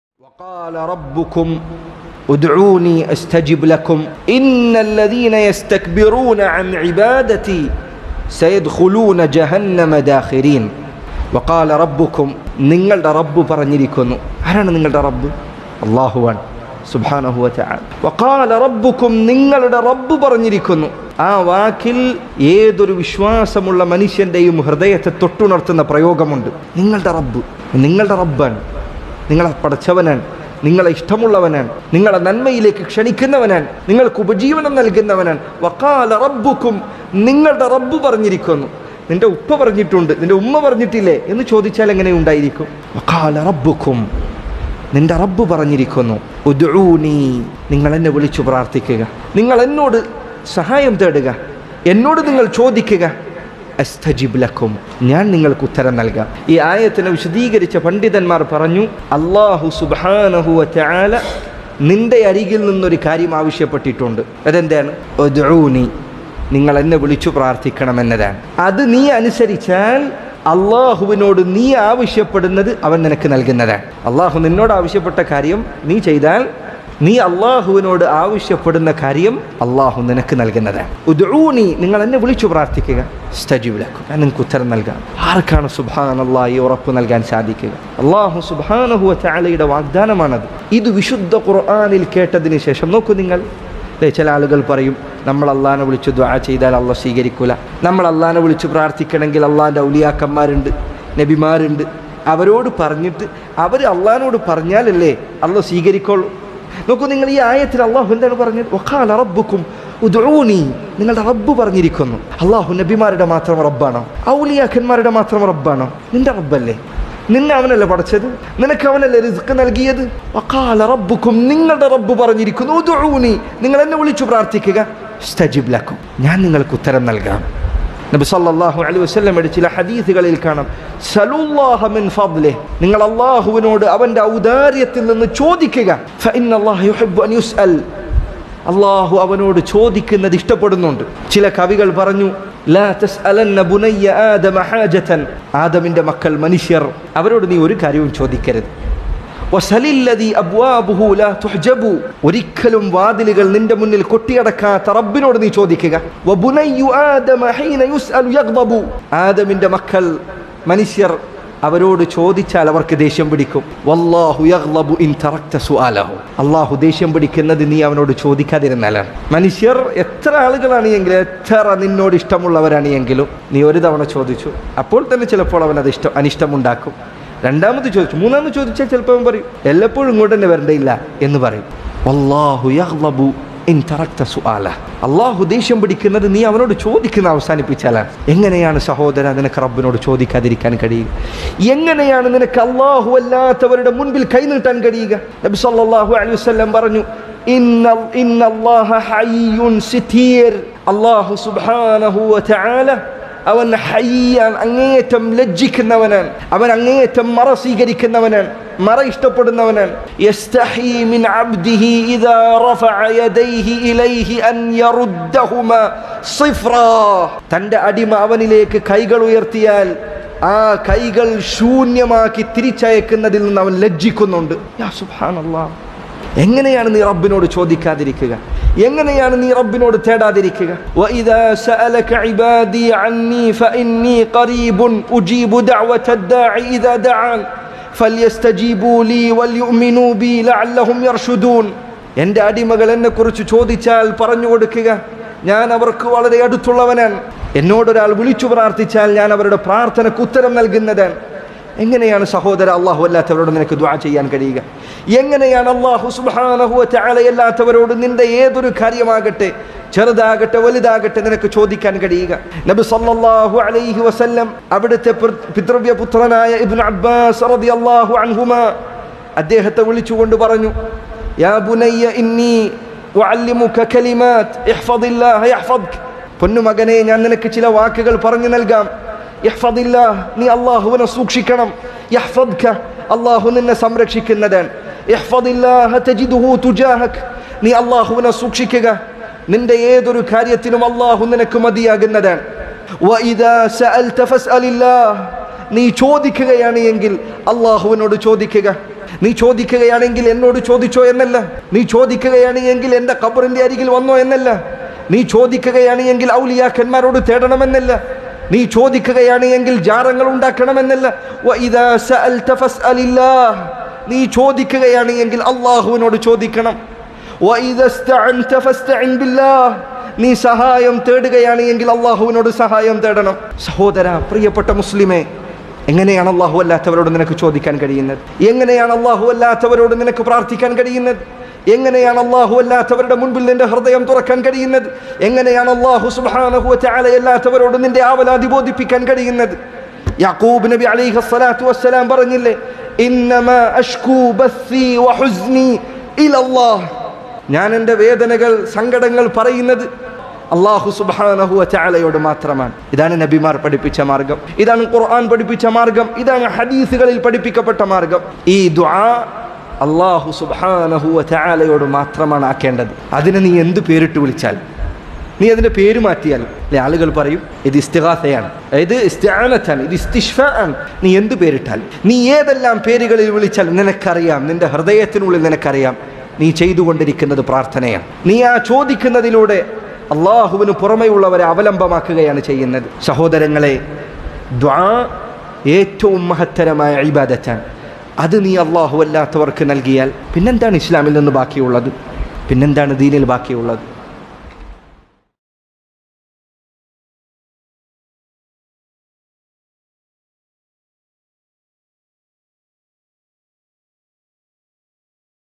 എങ്ങനെ നിനക്ക് അല്ലാഹുവല്ലാത്തവരോട് പ്രാർത്ഥിക്കാൻ കഴിയുന്നു..? | Abdul Muhsin Aydeed
എങ്ങനെ നിനക്ക് അല്ലാഹുവല്ലാത്തവരോട് പ്രാർത്ഥിക്കാൻ കഴിയുന്നു..?

തുറന്ന ഹൃദയത്തോടെ ഈ ആയതുകൾ കേൾക്കുക; ഹദീഥുകൾ ശ്രദ്ധിക്കുക. ശേഷം സ്വന്തത്തോട് ചോദിക്കുക: എങ്ങനെയാണ് എനിക്ക് അല്ലാഹുവല്ലാത്തവരോട് പ്രാർത്ഥിക്കാനോ സഹായം തേടാനോ കഴിയുക..?!

6.45 Minutes